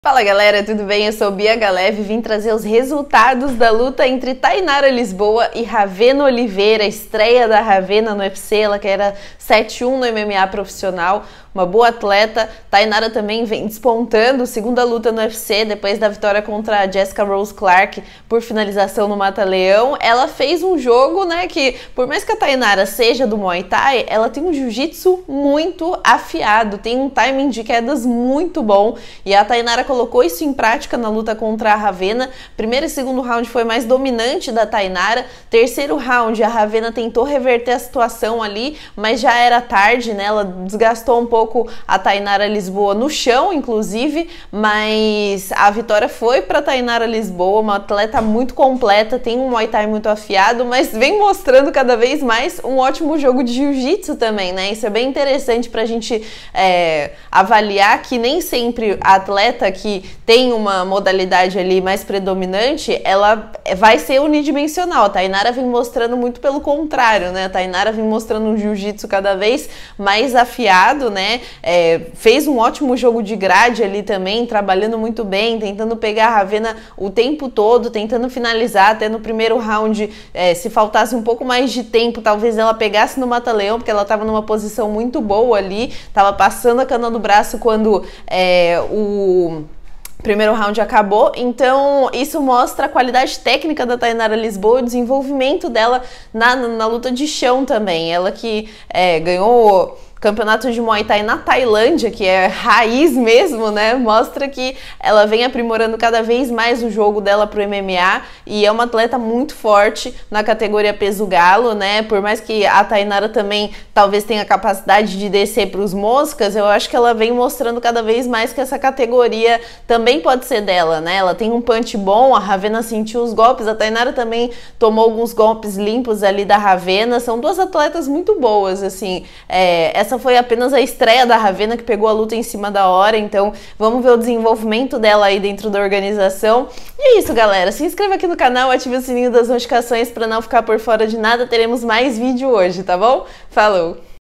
ENTREGARAM TUDO! Tainara Lisboa mostra jogo afiado contra Ravena Oliveira no UFC | Resultados
Vídeo de resultados sobre Tainara Lisboa e Ravena Oliveira no UFC Vegas 81.

Obs: as imagens da luta são de direitos exclusivos do UFC.

Foto de Capa: Chris Unger